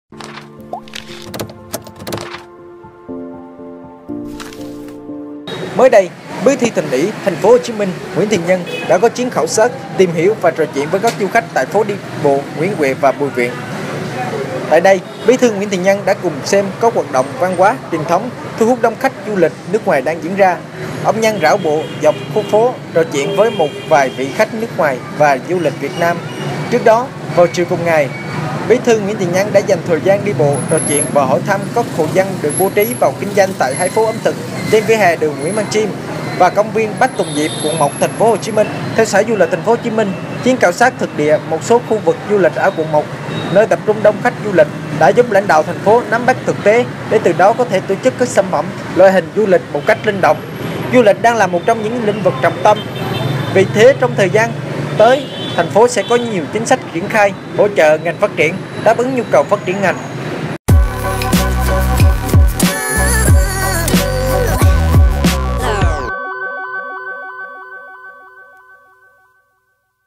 Bí thư Thành ủy Nguyễn Thiện Nhân trò chuyện 2 thứ tiếng với du khách ở phố đi bộ Bùi Viện
Kênh thông tin VietNam News là kênh thông tin hàng đầu, cập nhật liên tục các vấn đề xã hội, giải trí, ẩm thực...